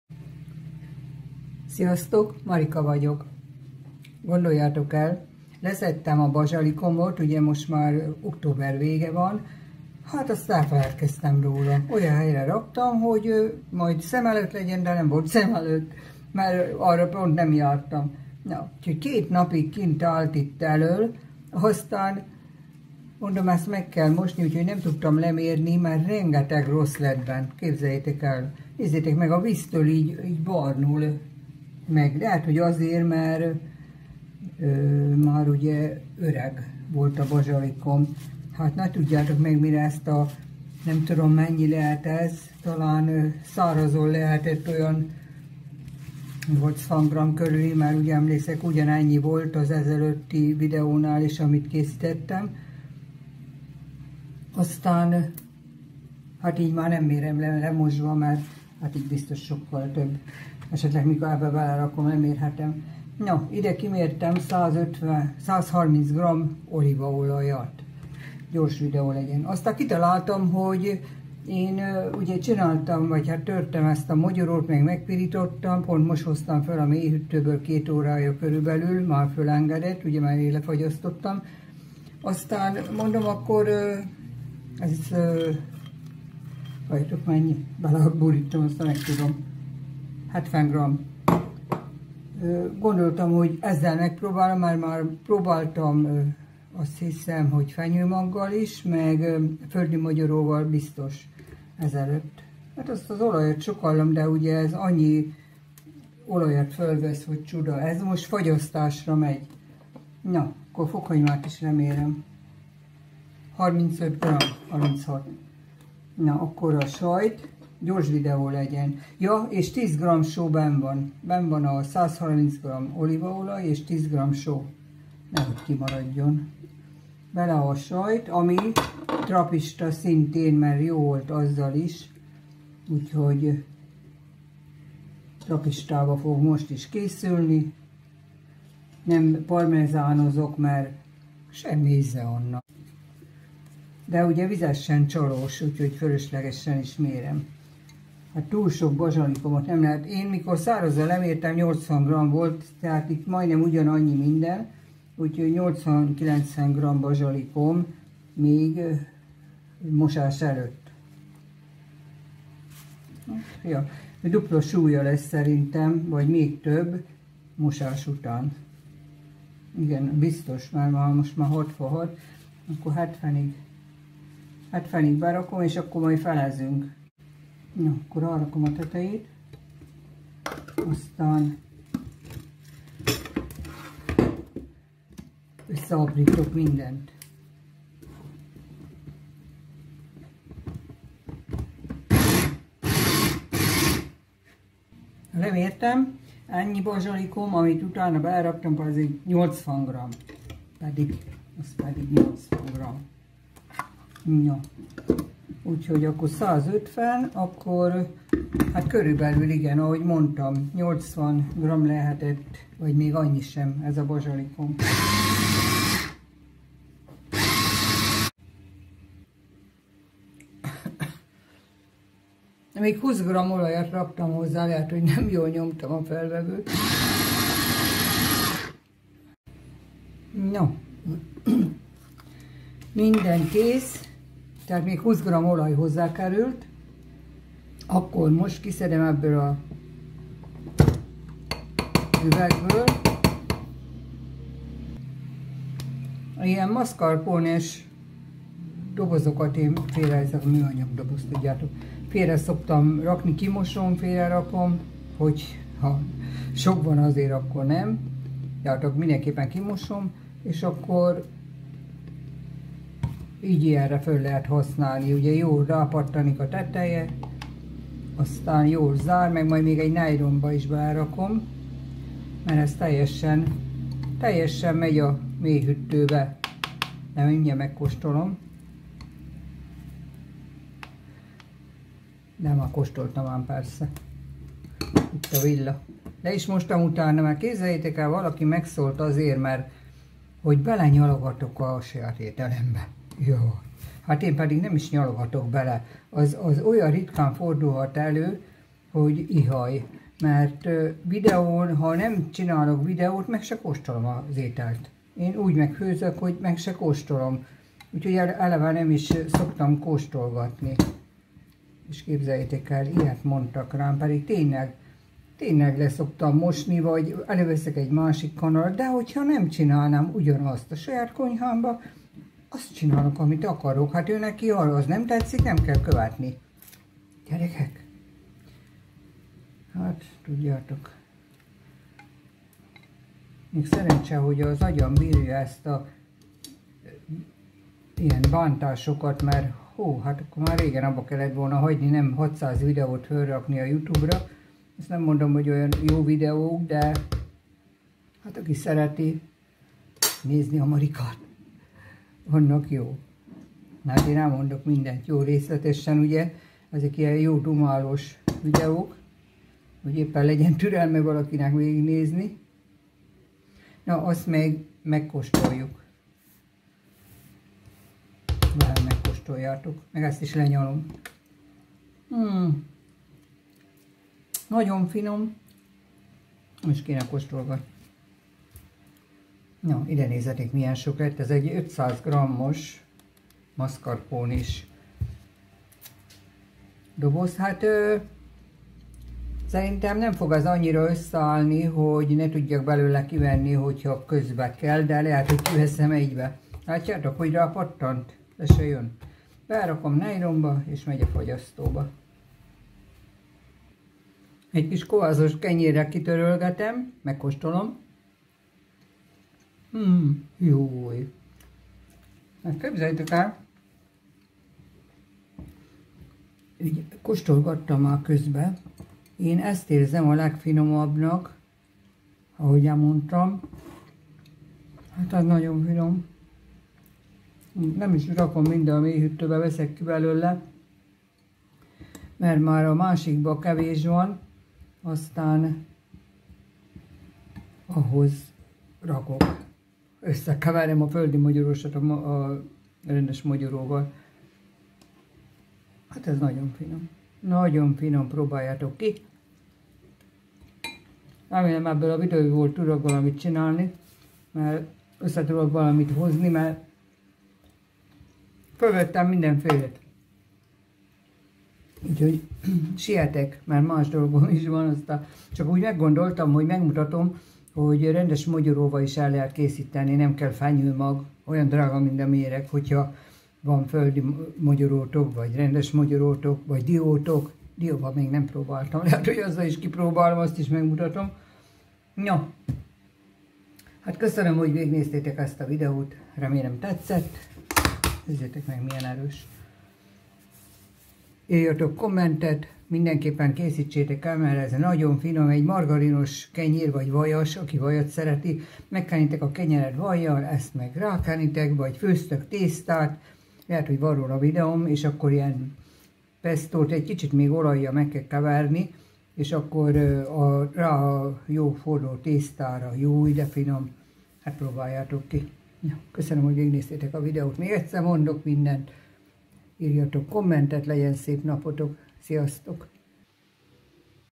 Mogyoróval készült pesztó a legfinomabb, szerintem. (Pesto )
Állítólag egy hétig lehet tárolni hűtőben, de nem értem miért. Én több hétig eszem, semmi bajom tőle. Döntsétek el Magatok mit vállaltok be. Minden esetre a hűtő 10 fok alatt legyen. ♥
150 gr. olívaolaj
70 gr. mogyoró, pirított
35 gr. fokhagyma
10 gr. só
80 gr. trappista
80 gr. bazsalikom , még mosás előtt